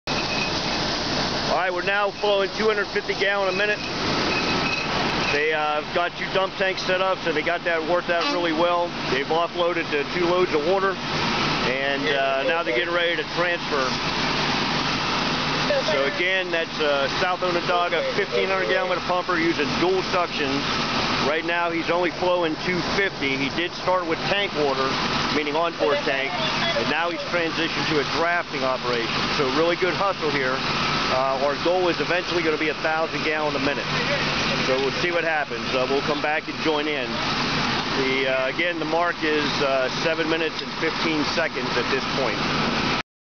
Part 3 - Rural Water Supply Drill - South Onondaga, New York - August 2014
We rejoin the drill in South Onondaga at the 5-minute mark where they have started to flow 250 gpm. We are sorry about the blurred view - we had an issue with the automatic focus - but the audio is good and we did not want to miss this part of the drill. The video clip is narrated by Mark Davis of GBW Associates, LLC.